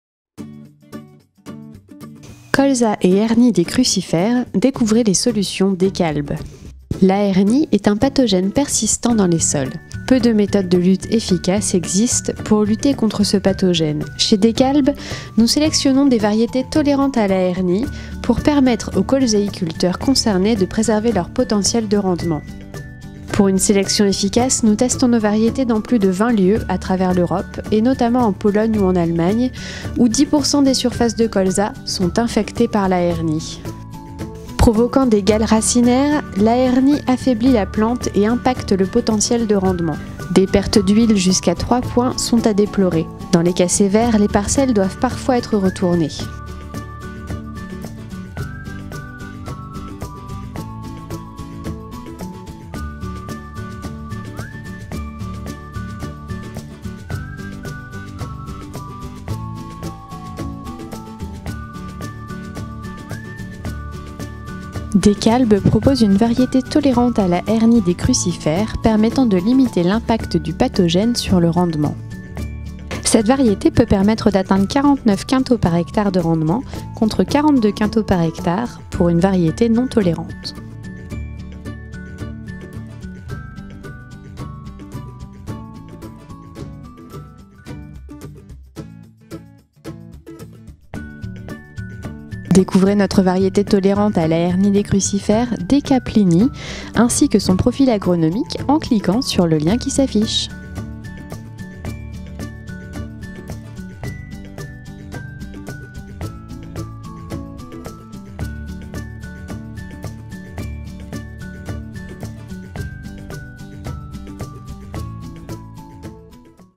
DK PLINY : même pas peur de la hernie des crucifères
Découvrez DK PLINY : nouvelle variété tolérante à la hernie des crucifères permettant de préserver le potentiel dans les zones touchées par la maladie